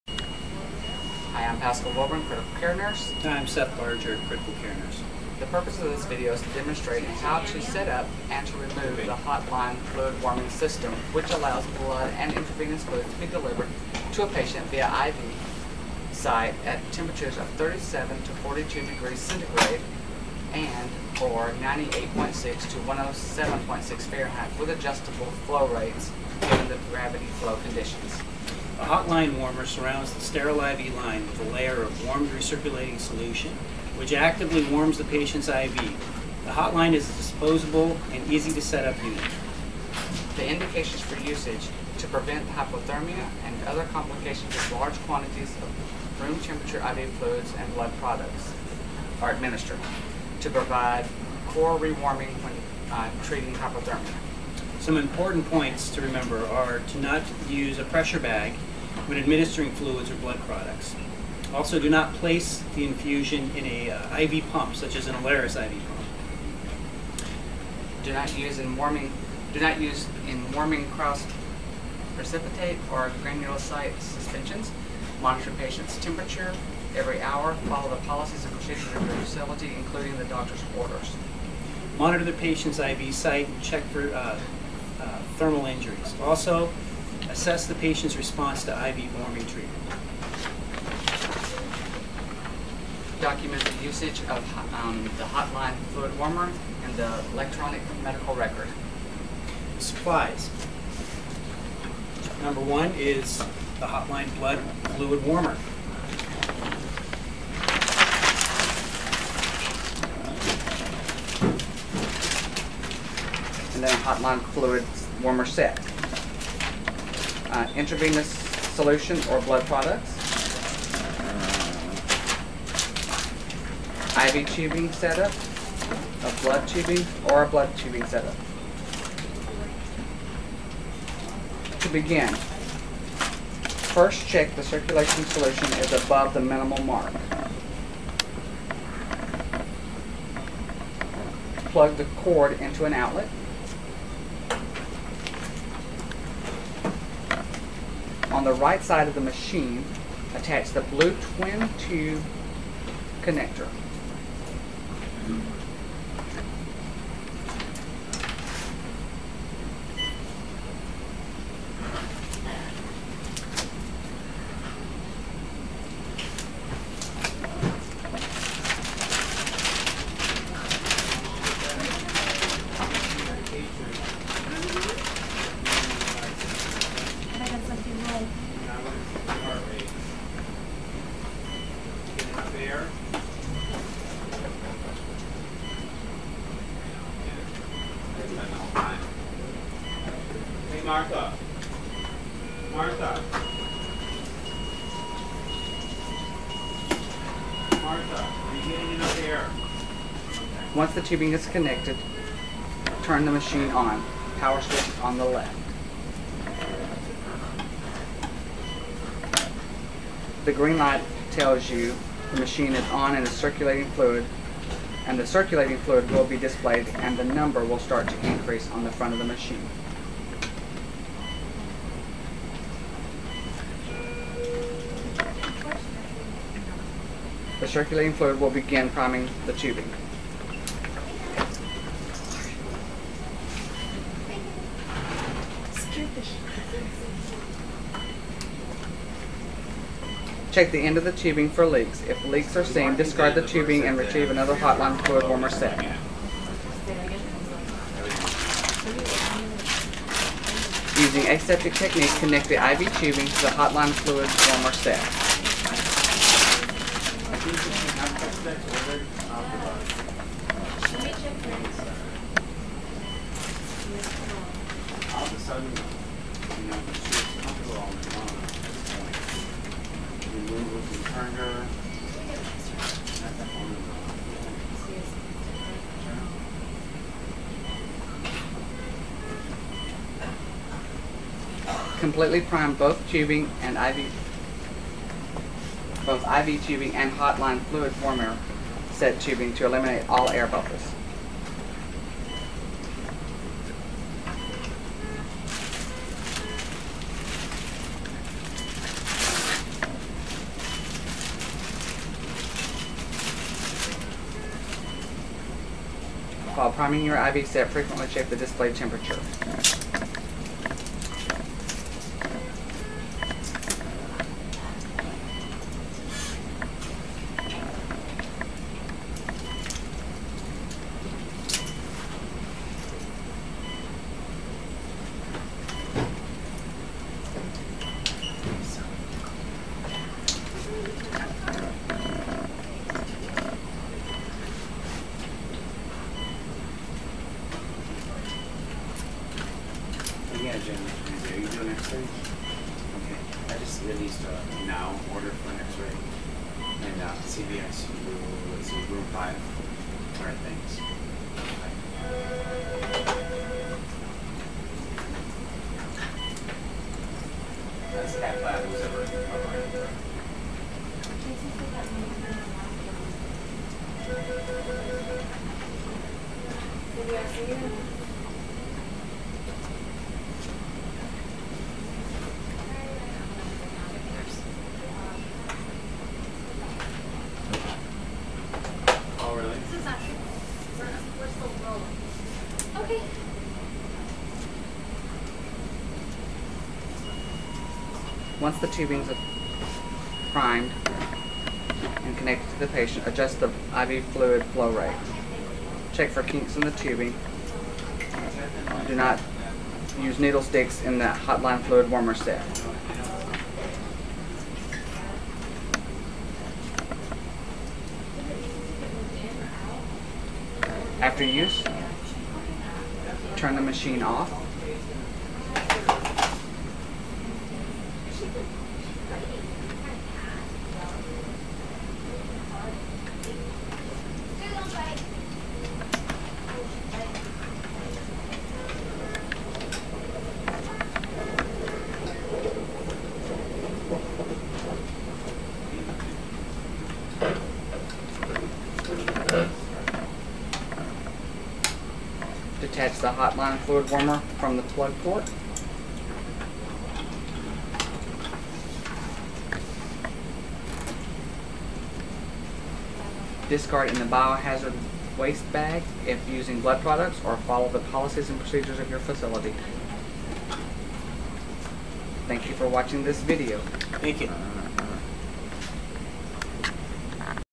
Hotline Blood and Fluid Warmer Setup by Nurses
Critical Care Nurses demonstrating how to setup Hotline blood and fluid warmer